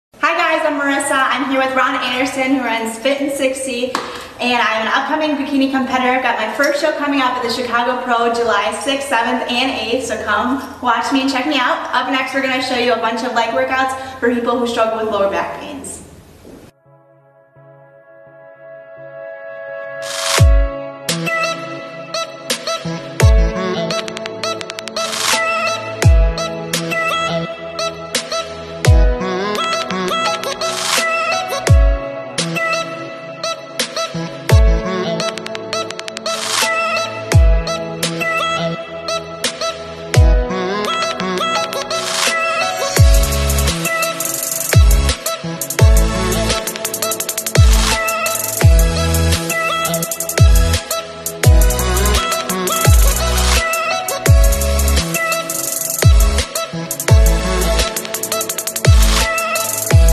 Leg Workout For Those With A Bad Lower Back | Throwback | Fit In 60 With Ron Anderson By Finetix®
Here is an oldie but a goodie.  This was how Fit In 60 started.  Check out the show - before it was what it is today.

Other companies owned by Finetix®